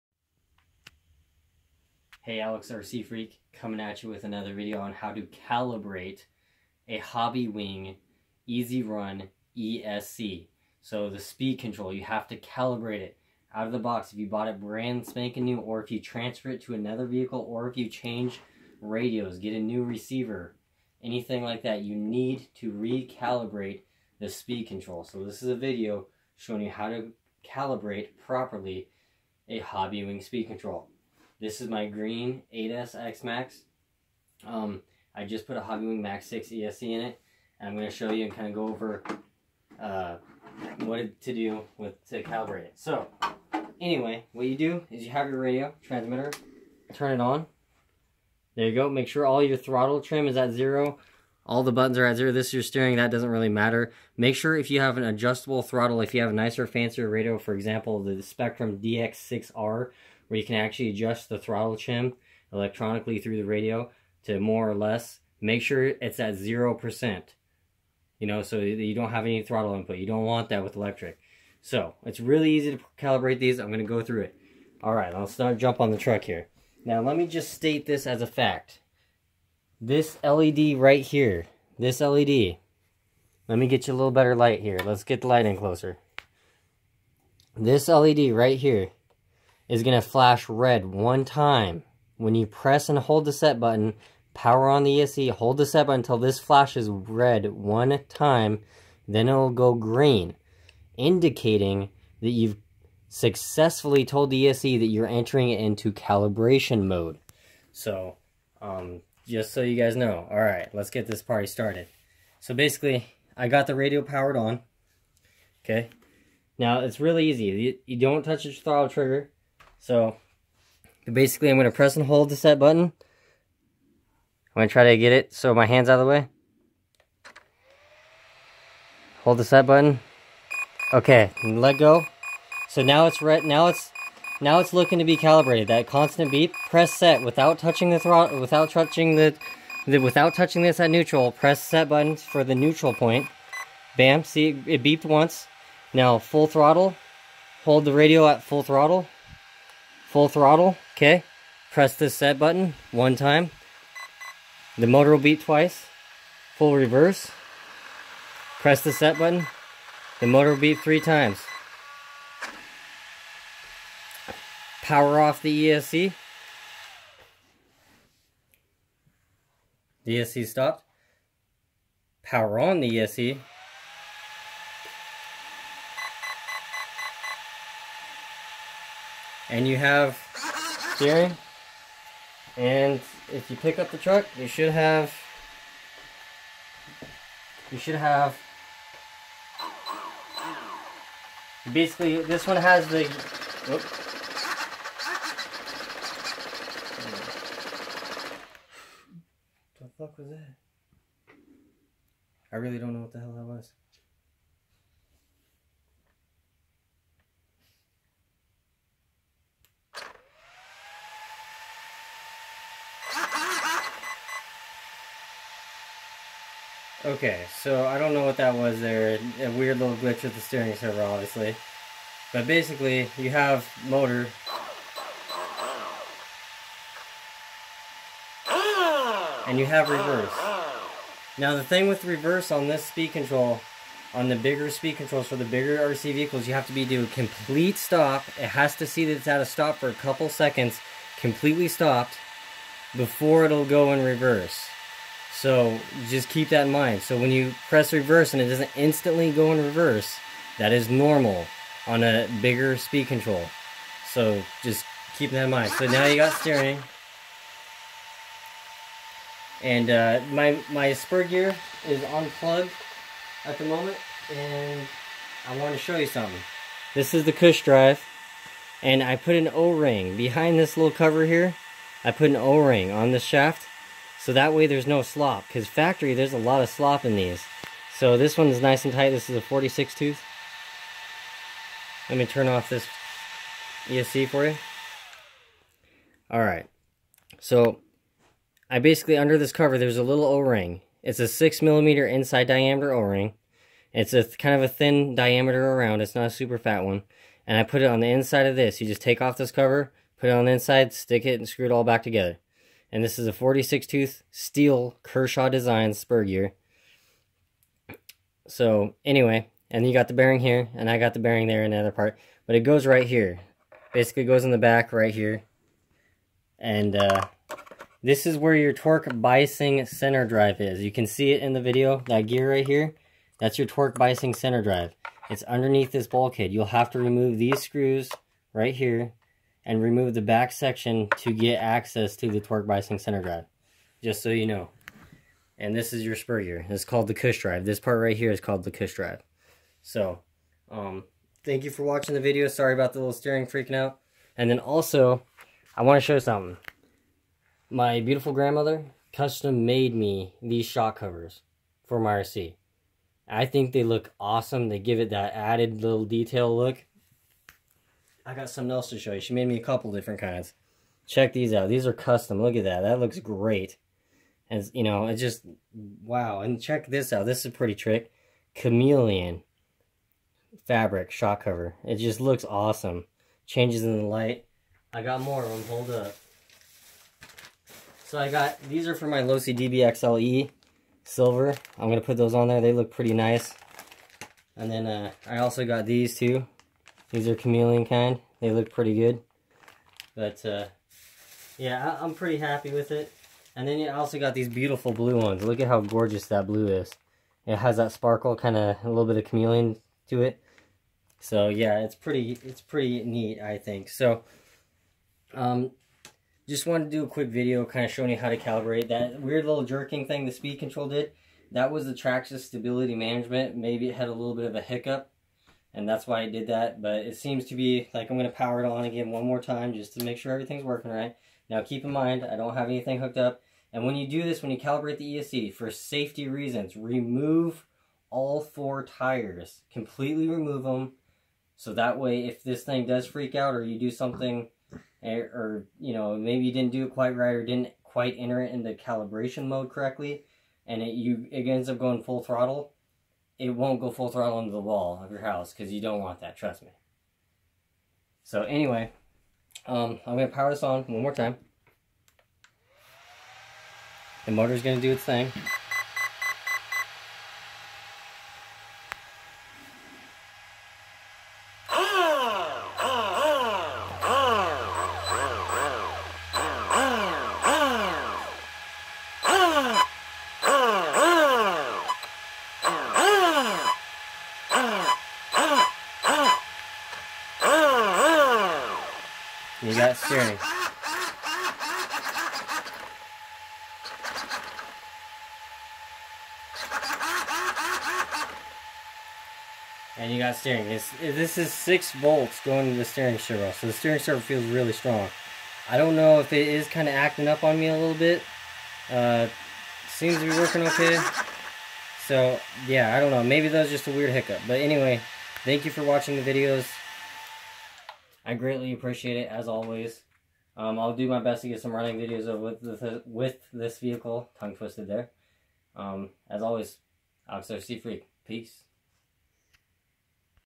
How to calibrate a hobbywing ESC in a Traxxas X maxx
This is a calibration video on how to calibrate a electronic speed control from hobbywing
This is  Demonstrated on my traxus X MAXX